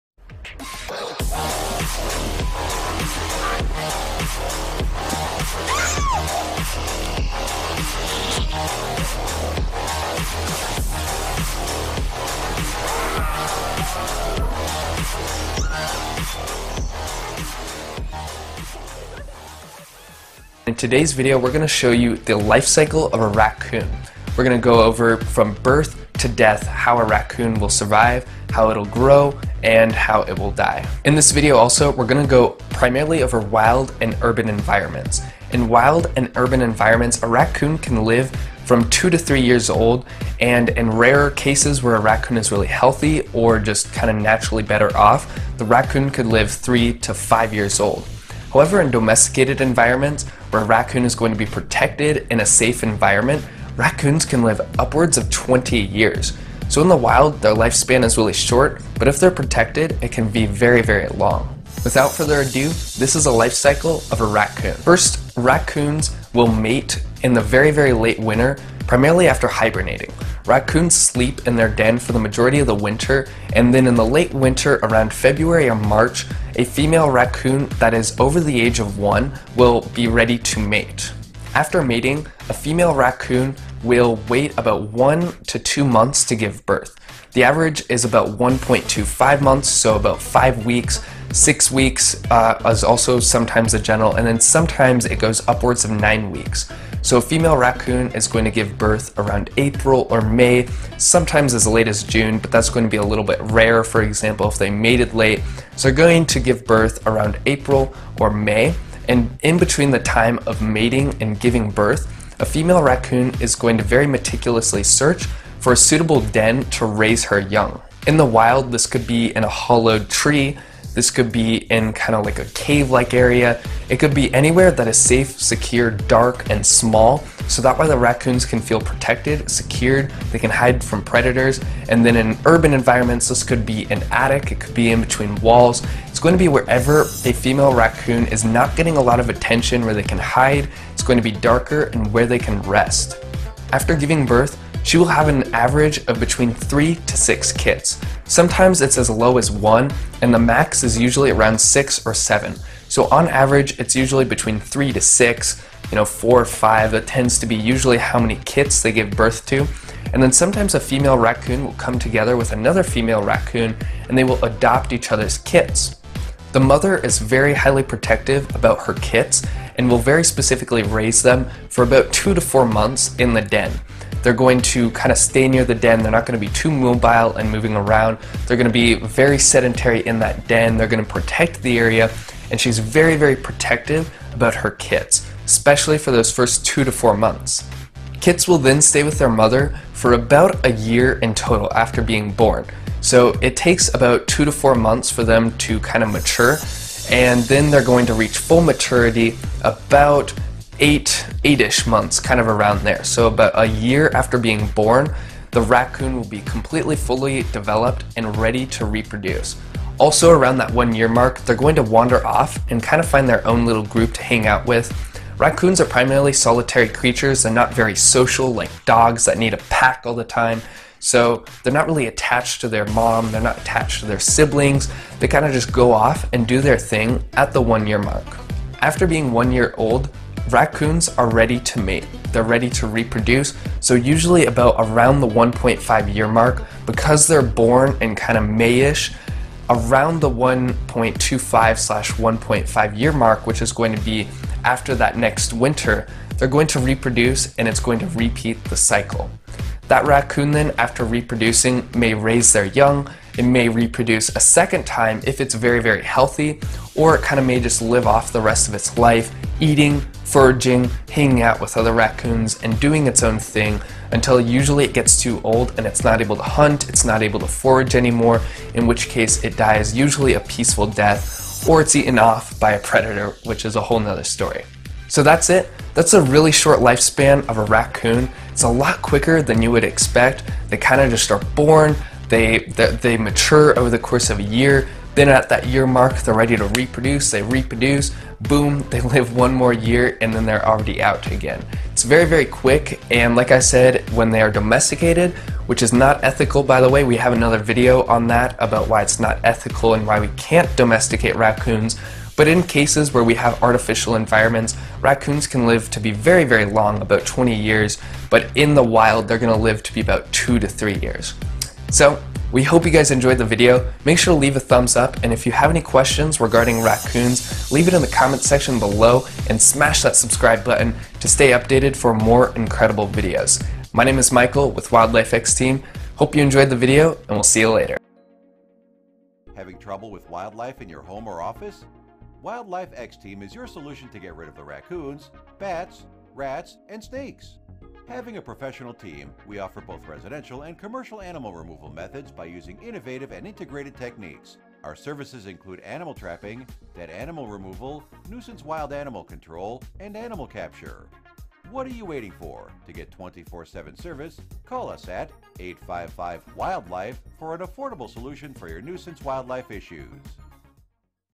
This is the Life Cycle of a Raccoon
Raccoons in the wild have a very short lifespan of about 3 years. The most they could probably live is 4-5 years if they're very healthy and in good circumstances. In safe, artificial environments, they can live upwards of 20 entire years! However, it's not ethical to try to tame a wild raccoon, and usually illegal to do so. This is the life cycle of your typical wild raccoon.

Thanks!
-Wildlife x Team International